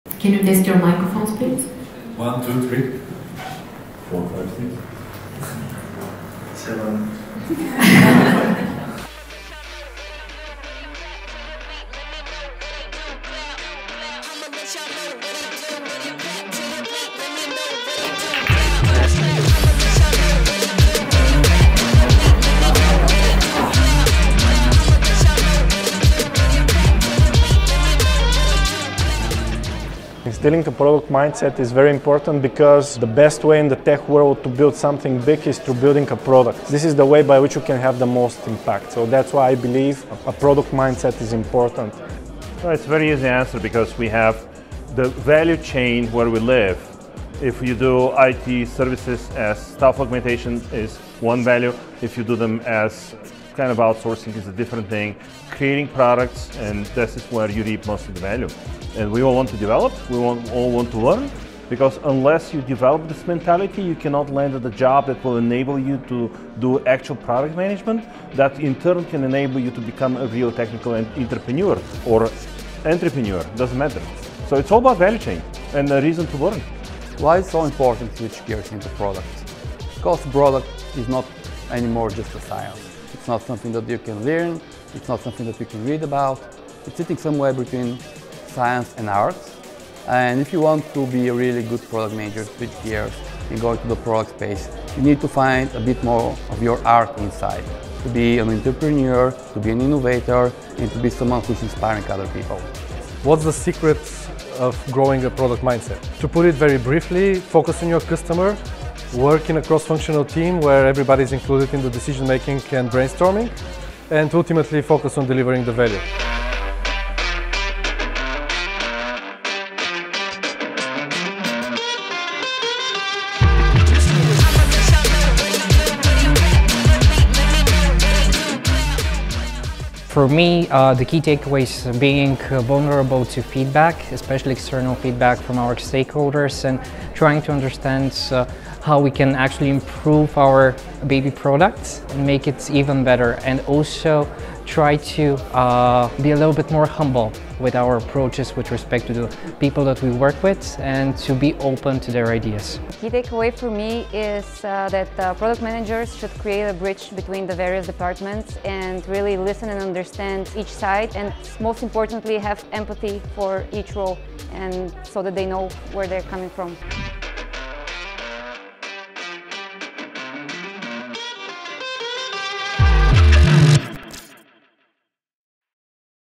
After Hours Vol 6: What’s the Secret Weapon To Growing a Product Mindset?
To succeed in the age of the digital economy, companies need to sleep, eat, and breathe product. Still, one of the most difficult tasks tech managers are faced with is growing product managers and instilling a product mindset within their companies.
But how do you switch gears between a project mindset to a product mindset? What to do if the Gantt chart rules the day, but you want to focus on delivering customer value instead?
After Hours Vol. 6 gathered Ivailo Ivanov - Sr. Director Product Management at CleverTap, Stanimir Dimitrov, Senior Manager, Fintech Products at Uber, Vesko Kolev, Founder & CEO at icanpreneur, and Andrey Bachvarov, General Manager at Anthill to find out what's the secret to  growing a product mindset.
Catch the highlights from the event.
Place: Office of Leanplum (a CleverTap company)